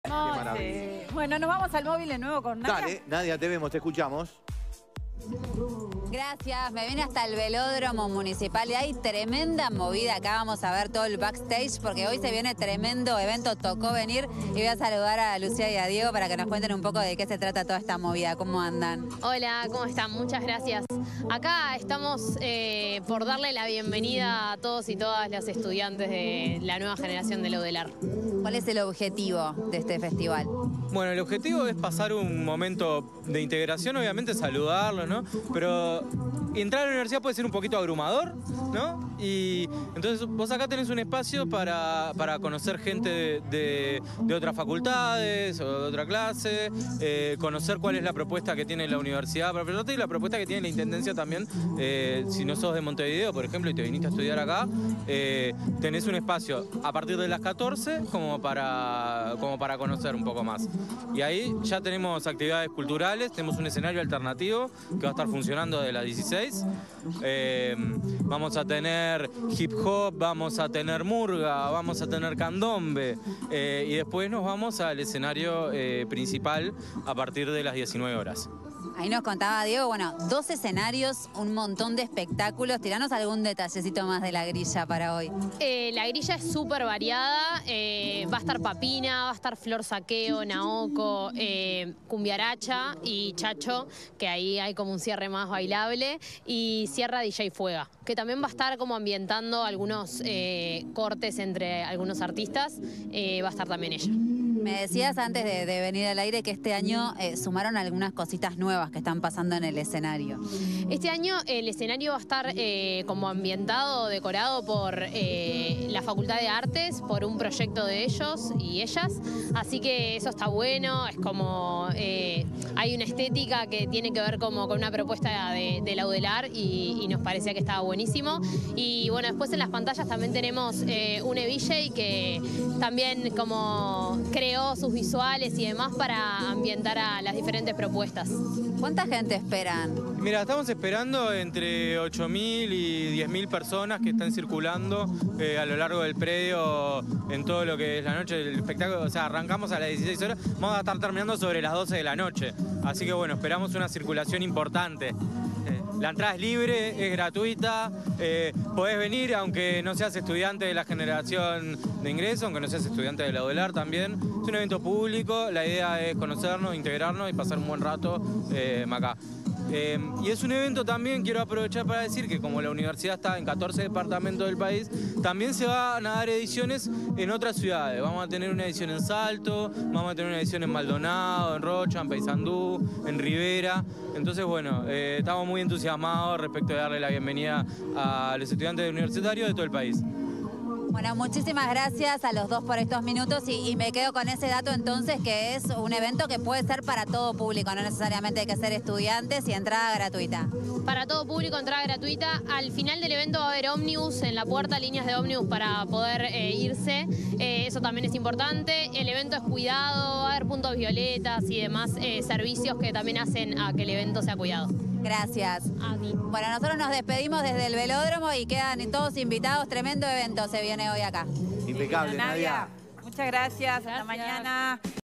Arrancó “Tocó venir 2024”: el evento de bienvenida a la nueva generación universitaria
Nadia Fumeiro converso con Lucía Acosta, directora de Juventud; y con Diego. El festival contará con stands, talleres, propuestas culturales y artísticas.